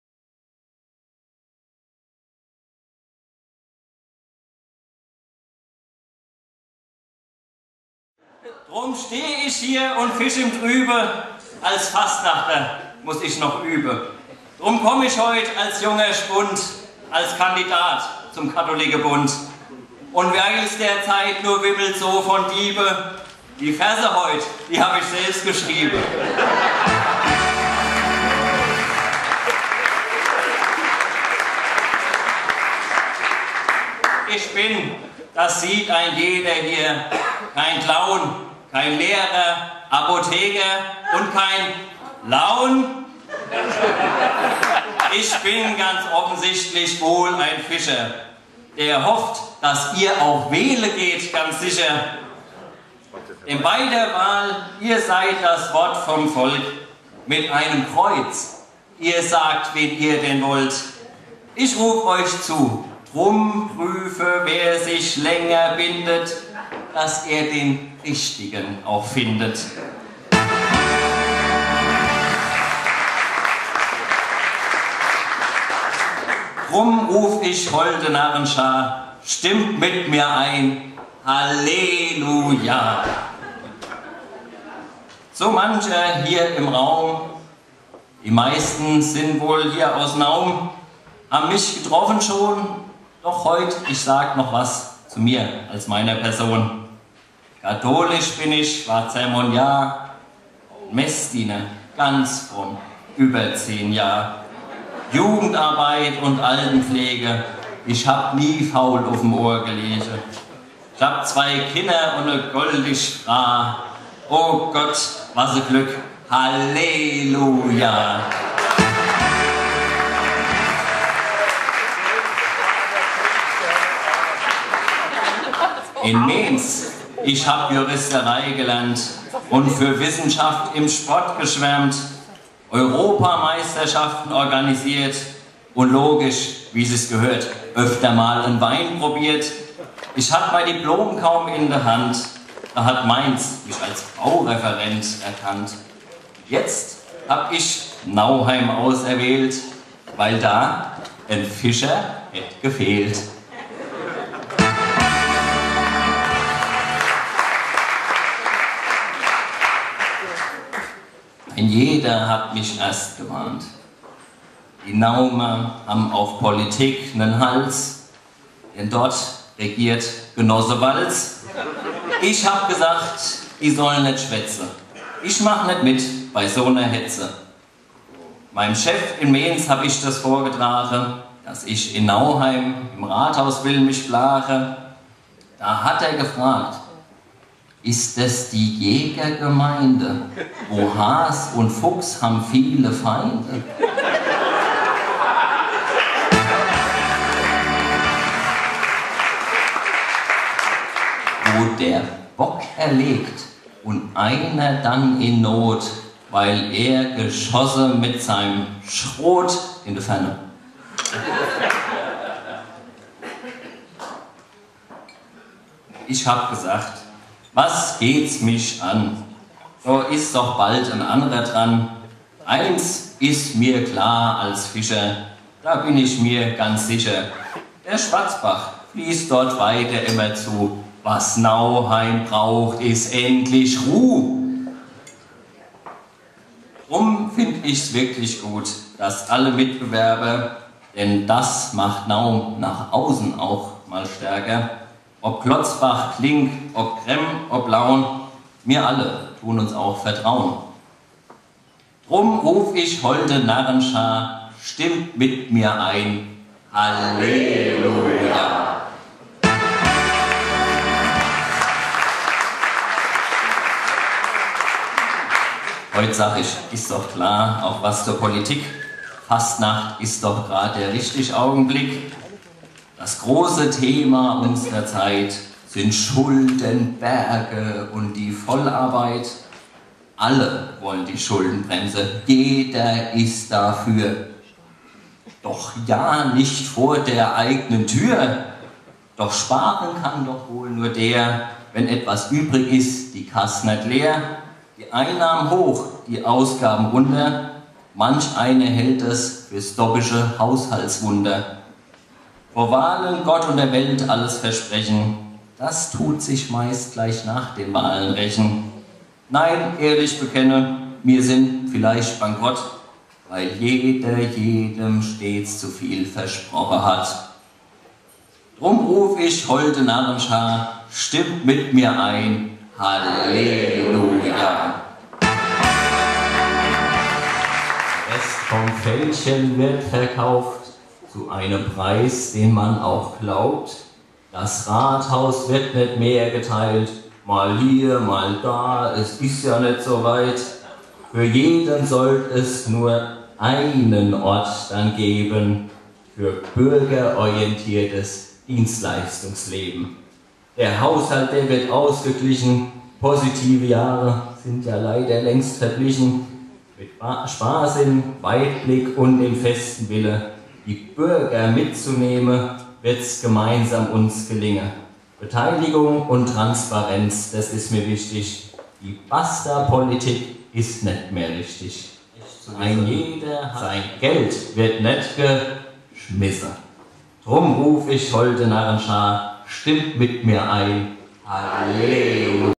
Büttenrede Jan Fischer (Auszug)
Bürgermeisterkandidat Jan Fischer bei KFK Nauheim